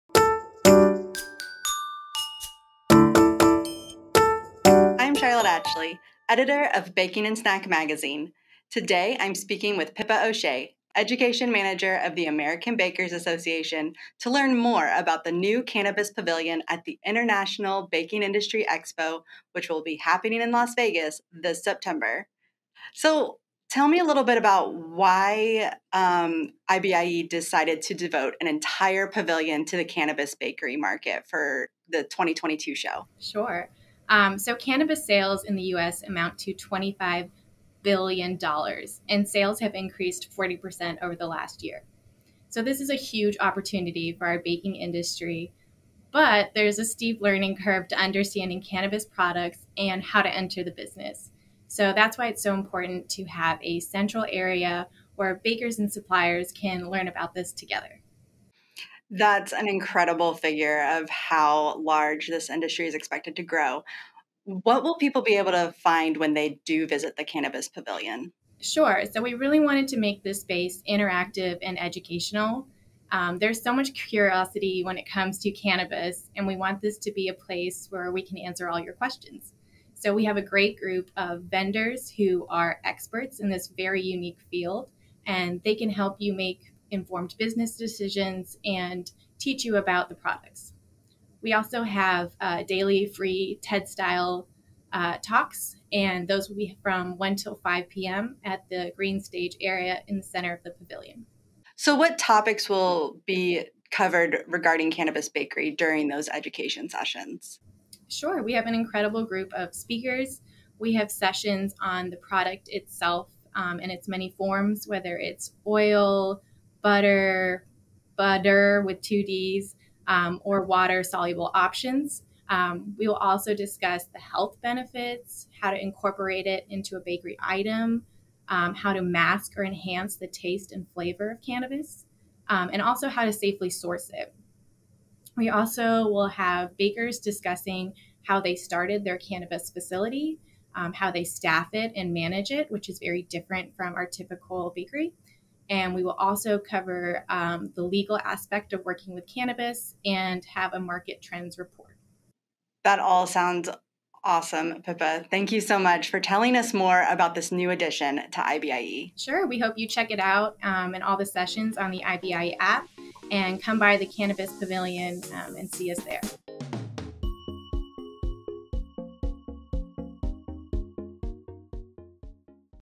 IBIE Hidden Gems: Cannabis Central
Ahead of the @IBIE2025, Charlotte Atchley, editor, Baking & Snack, spoke to Pippa O'Shea, education manager, @AmericanBakers, about Cannabis Central, which will provide  a centralized location for bakers to connect with suppliers of products and services in the cannabis baked goods space.